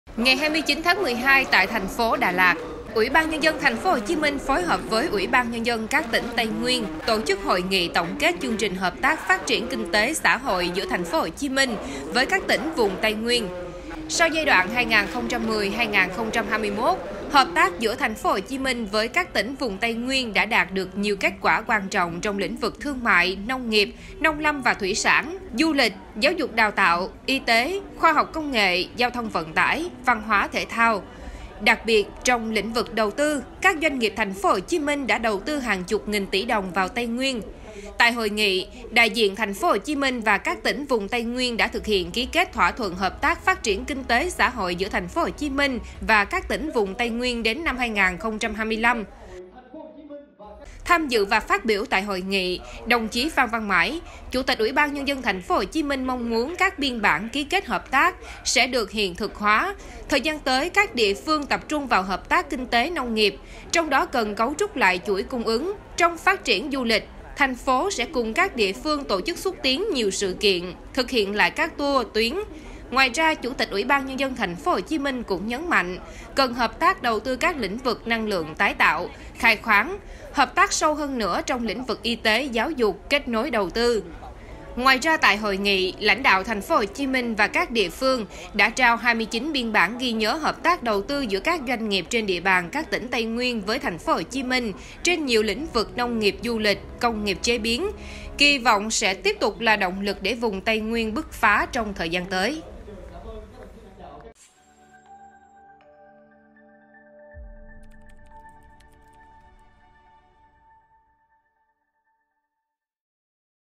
TPHCM ký kết nhiều hợp tác đầu tư, kỳ vọng vùng Tây Nguyên sẽ bứt phá
Ngày 29-12, tại TP Đà Lạt, UBND TPHCM phối hợp với UBND các tỉnh Tây Nguyên tổ chức Hội nghị tổng kết Chương trình hợp tác phát triển kinh tế - xã hội giữa TPHCM với các tỉnh vùng Tây Nguyên.